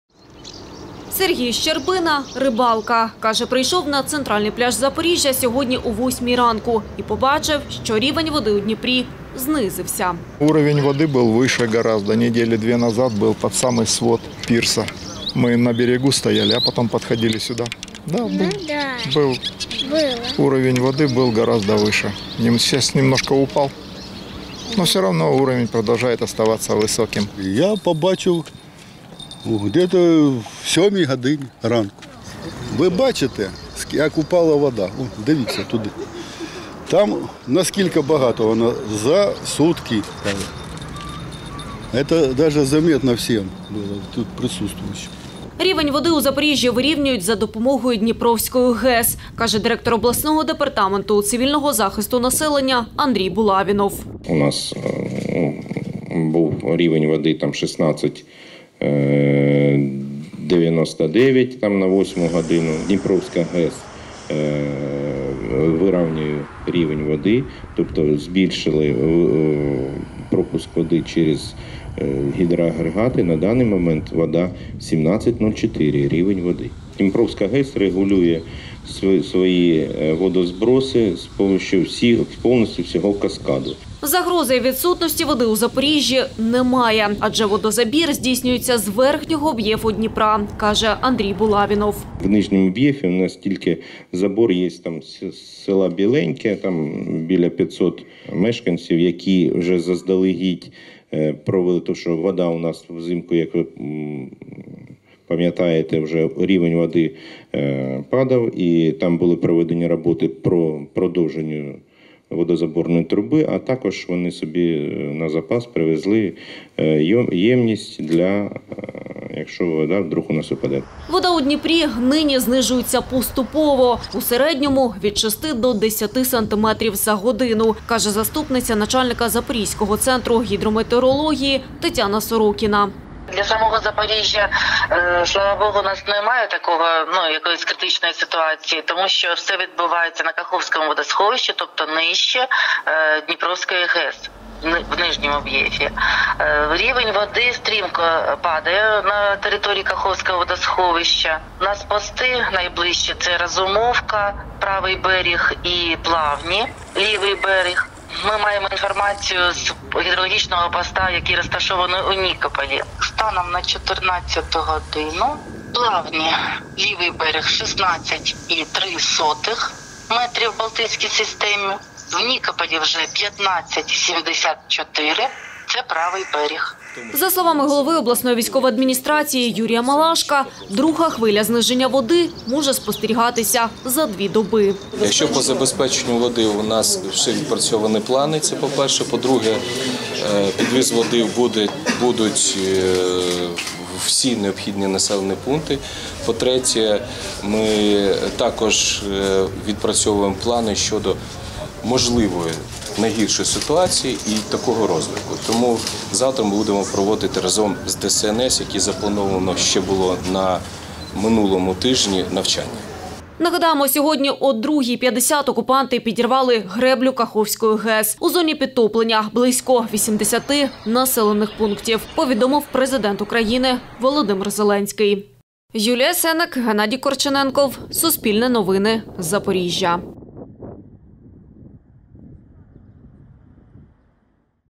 Наслідки підриву росіянами Каховської ГЕС для Запорізької області | Новини
Сьогодні, 6 червня, армія РФ підірвала Каховську ГЕС, внаслідок чого різко почав знижуватися рівень води. Чи загрожує це ядерній безпеці Запорізької АЕС та як буде розвиватися ситуація надалі, — читайте у нашому матеріалі.